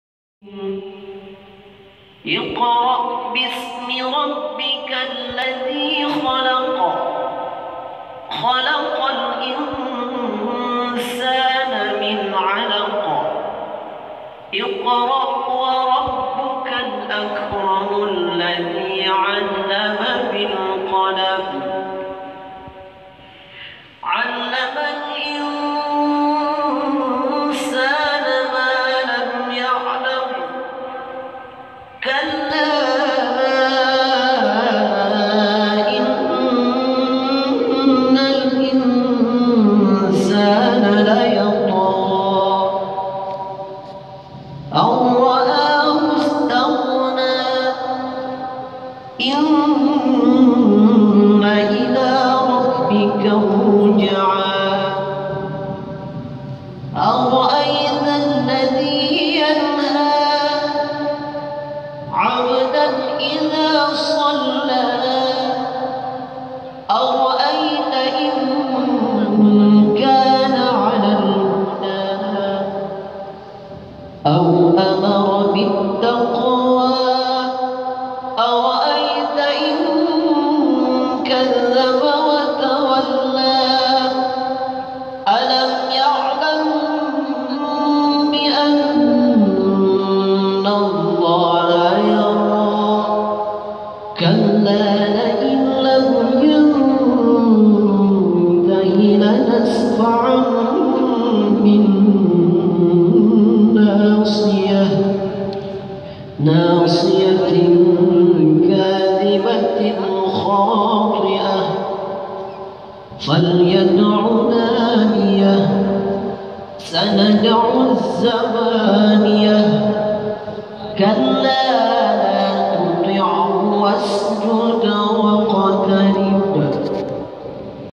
Merdunya Bacaan Surah AL-ALAQ | bikin hati luluh
video ini diambil menjelang pelaksanaan shalat jumat tahun 2017. terlihat sang Qori sedang membacakan surat-surat pendek menjelang diselenggarakannya ibadah shalat jum'at di Masjid Al-Jamiah UIN SUSKA Riau. sementara kalangan jamaah dari kalangan Dosen dan Mahasiswa terlihat bersiap siap dalam tata tertib penyelenggaraan sholat jumat.